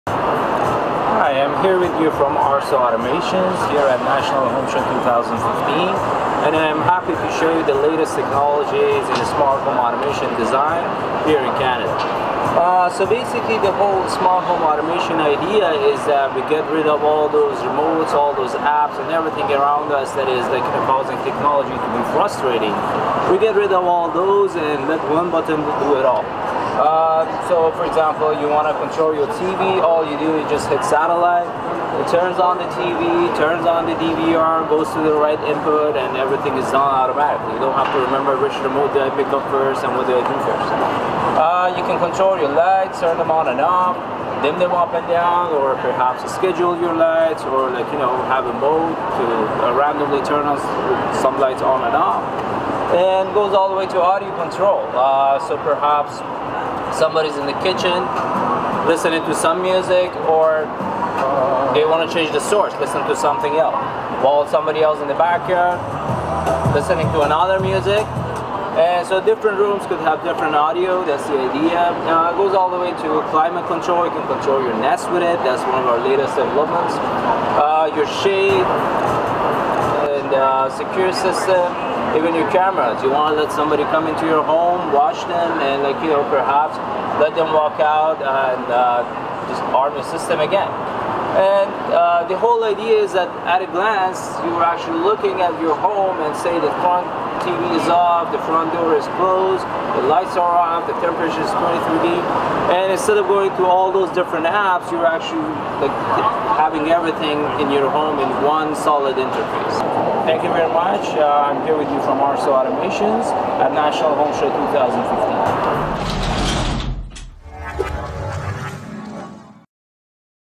ARSO Smart Home Demo
This video is a quick demo of ARSO's model designed for Canada's national home show. The components of this system as well as its interface would be different for every house as it is all custom programming.